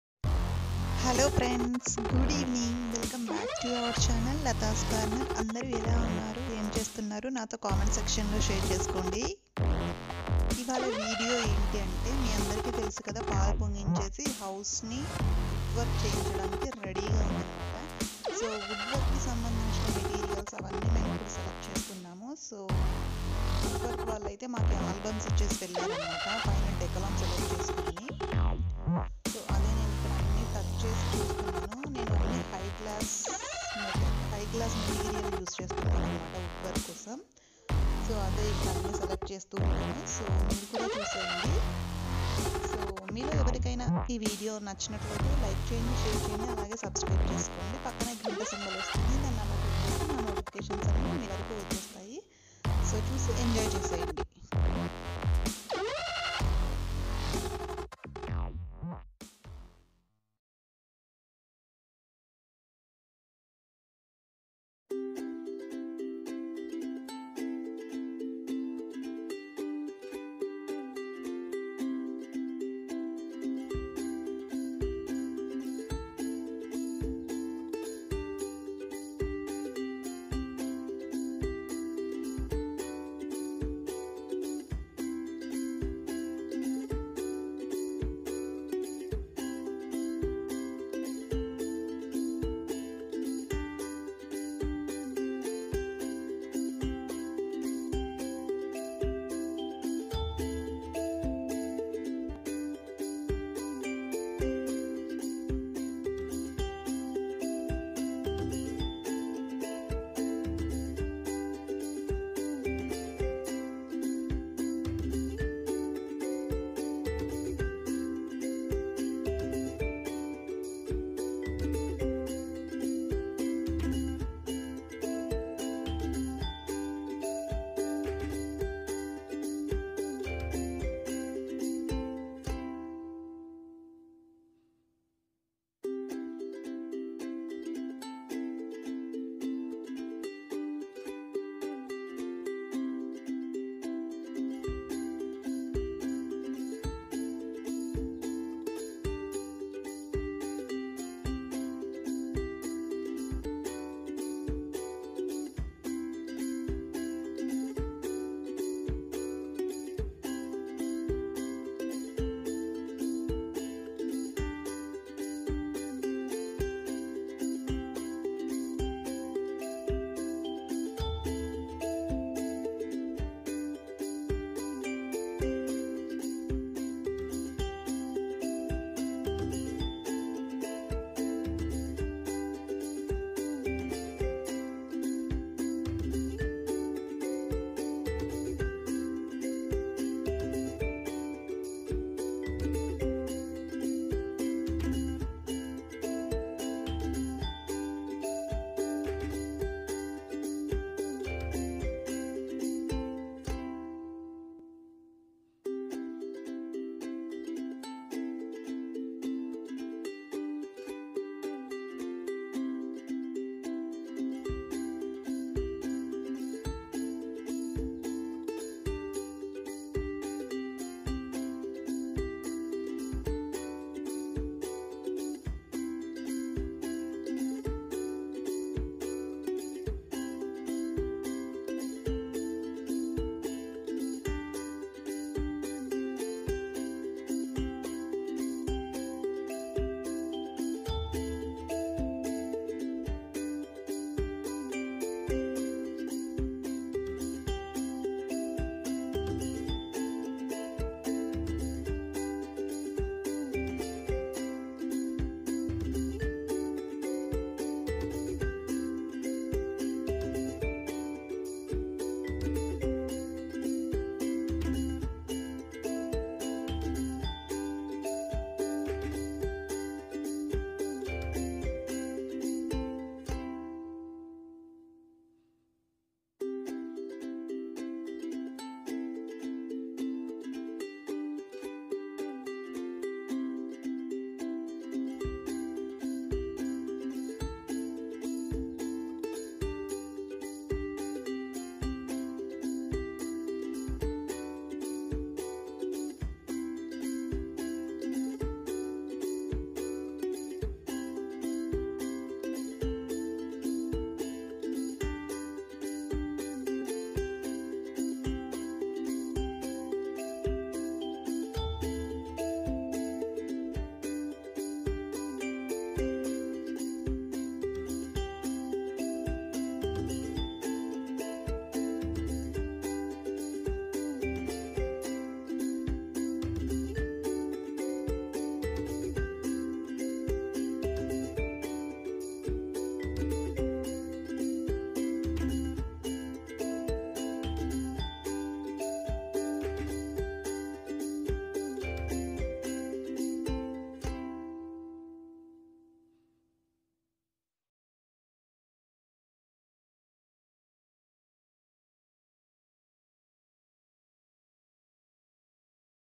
మా కొత్త ఇంటి కి wood work selection start చేసాం
This video is wood work selection of my new house